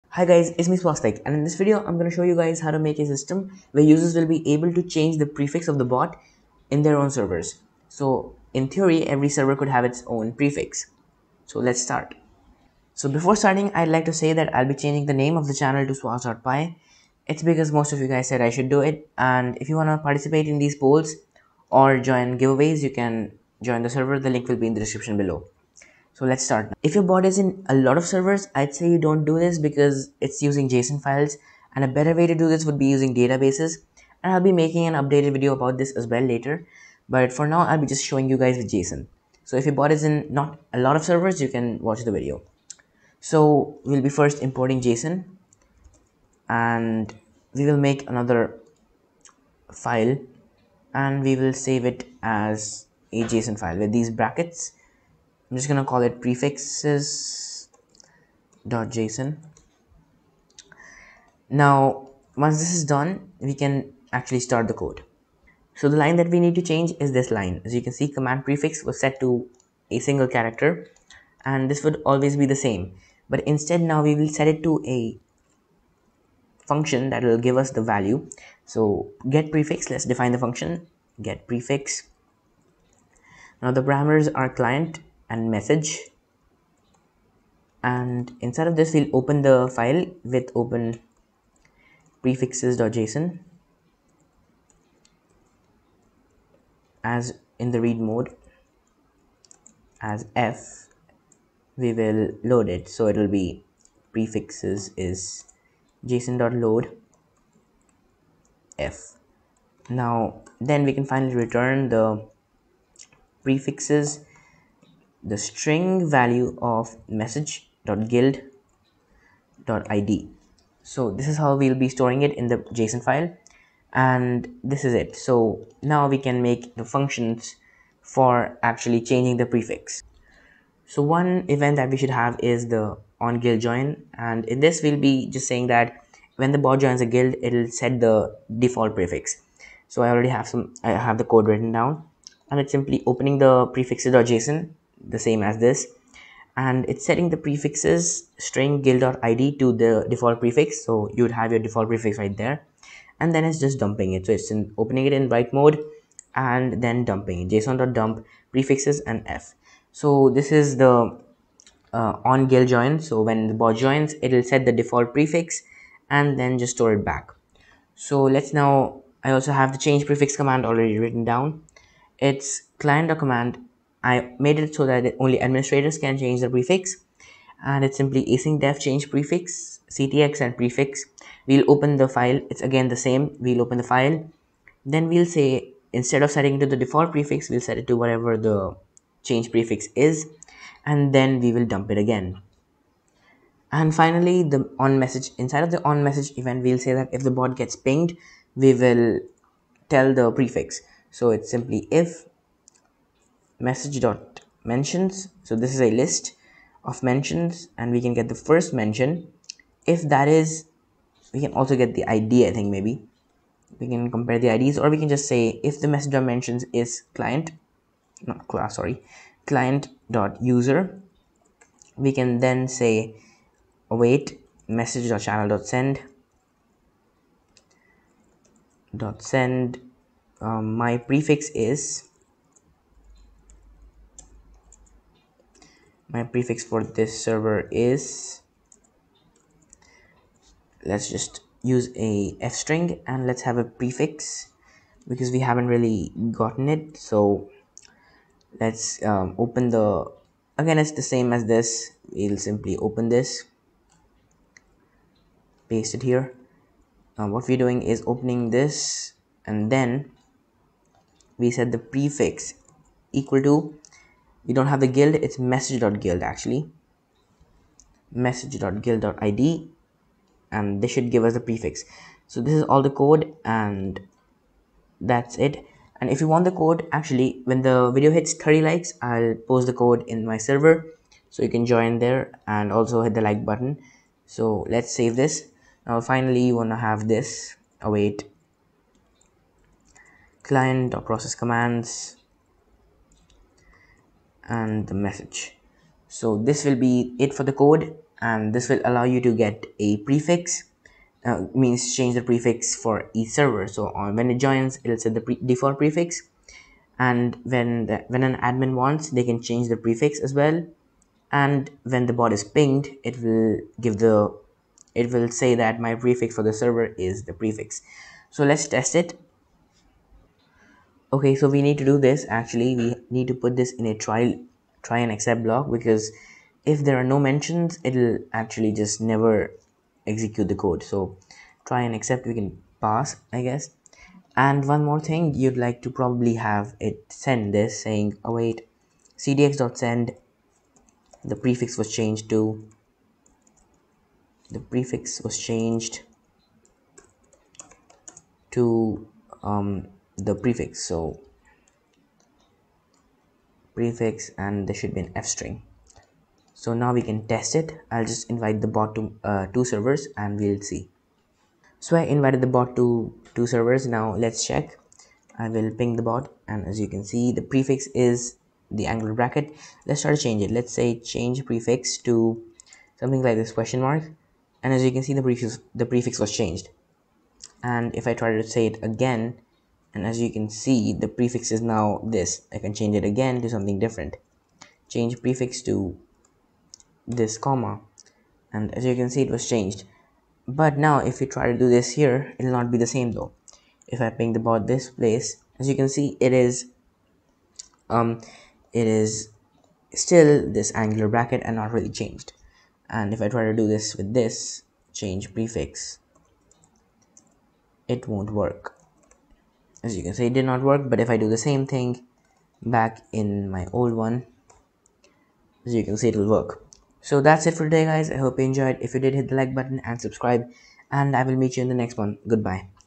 Per Server Custom Prefixes in Discord.py | Making a discord bot Part: 9
In this video, we will learn how to make a system where users can set custom prefixes for a Discord bot in python using discord.py (rewrite) in 2020.

If you have any suggestions for future videos/doubts, leave it in the comments below.

Queries solved in the video:
1) How to set a Custom Prefix with discord,py
2) How to have Per Server Prefixes in Discord.py